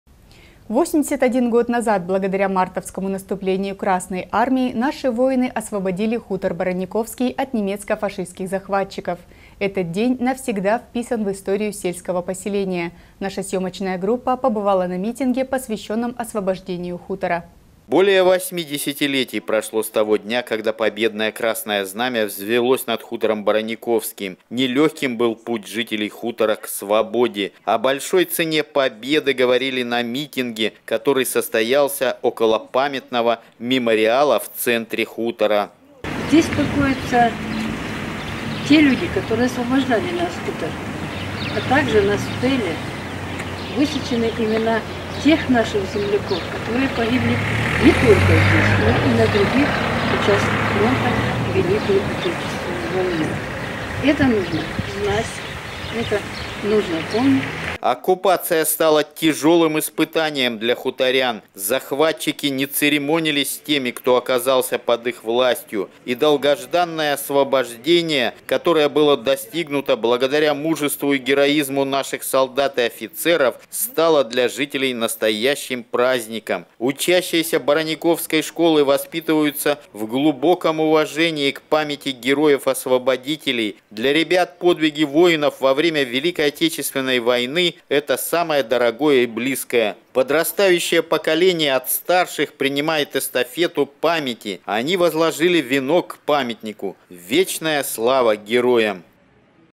Памятный день
81 год назад благодаря мартовскому наступлению Красной Армии наши воины освободили хутор Бараниковский от немецко - фашистских захватчиков. Этот день навсегда вписан в историю сельского поселения. Наша съемочная группа побывала на митинге, посвященном освобождению хутора